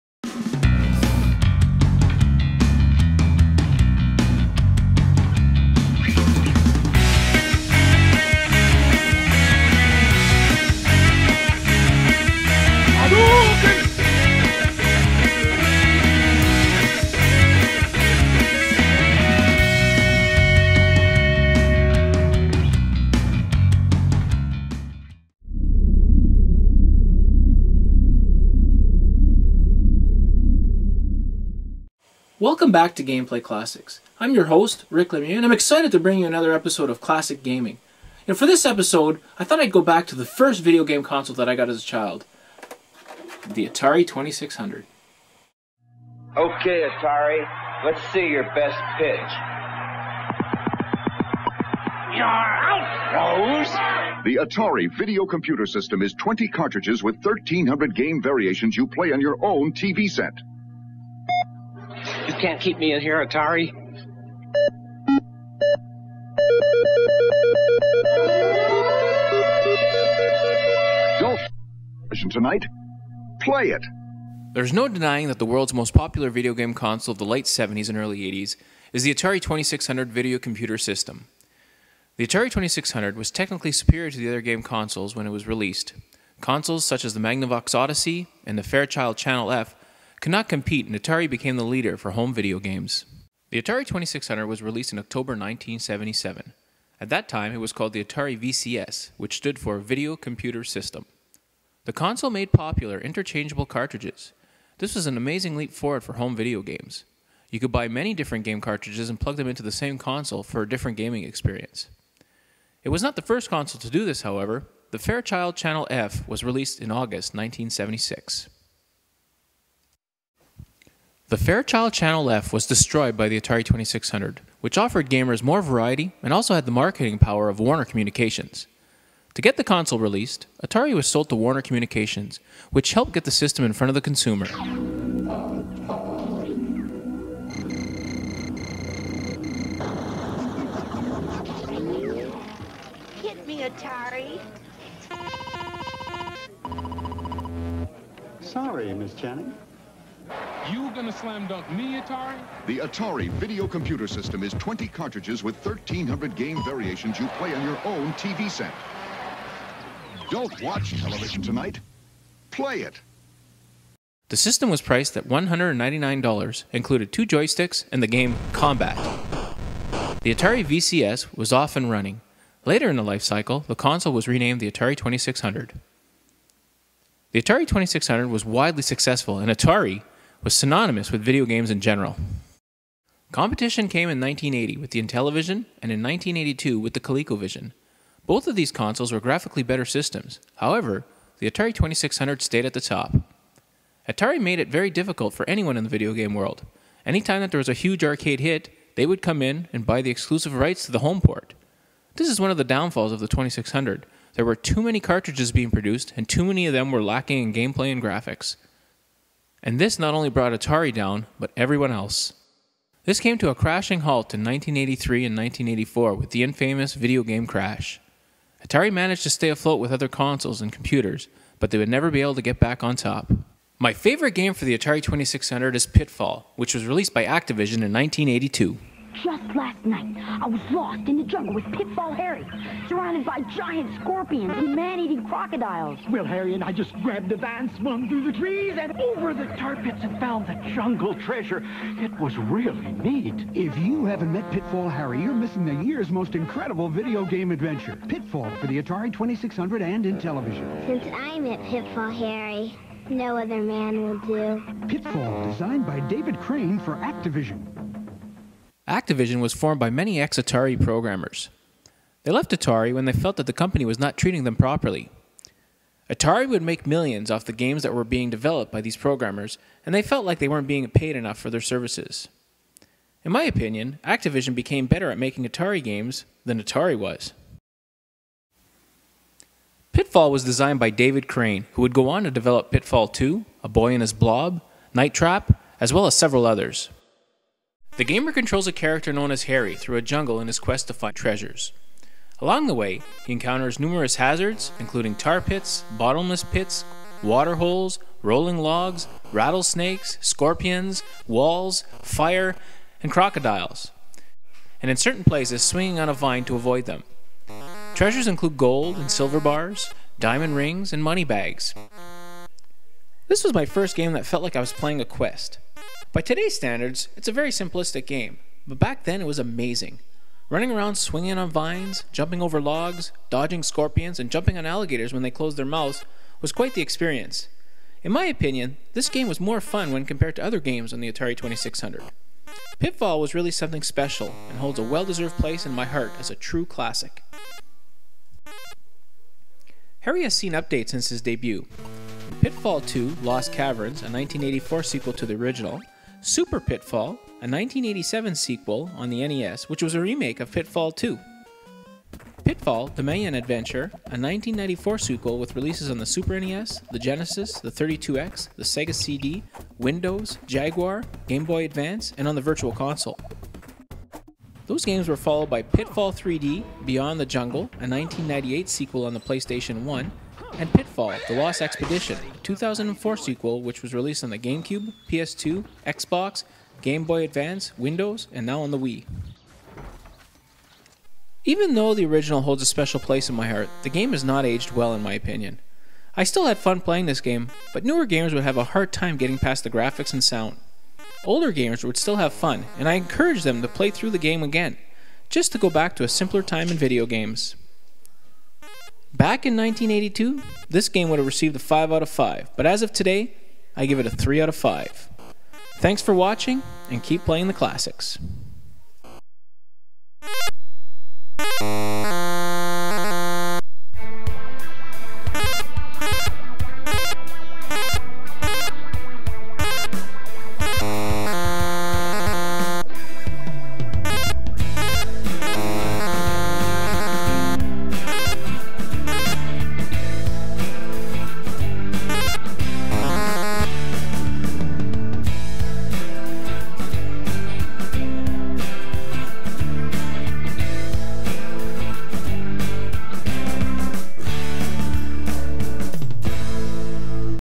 Gameplay Classics - Episode 3 - Atari 2600 and Pitfall!
A brief history on the Atari 2600 and a review of Pitfall!